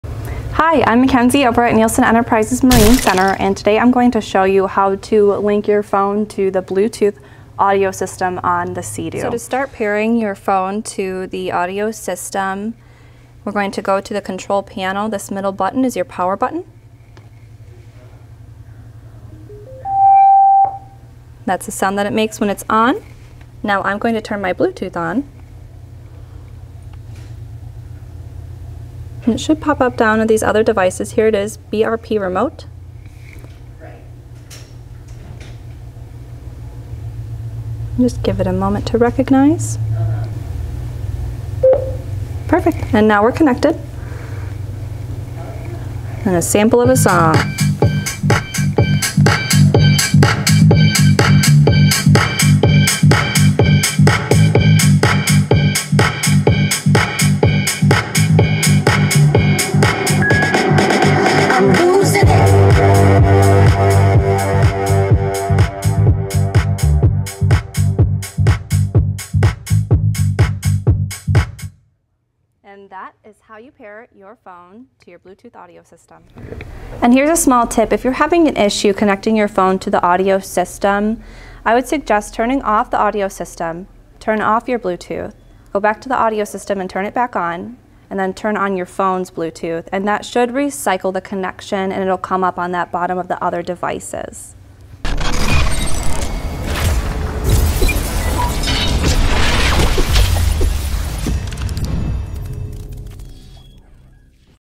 HOW TO: Pair your phone a Sea-Doo BRP Audio System
Having an issue, or just not sure how?  Watch Mackenzie give you some tips and troubleshooting tips on syncing your phone to your Sea-Doo Watercraft.
Enjoy that amazing sound!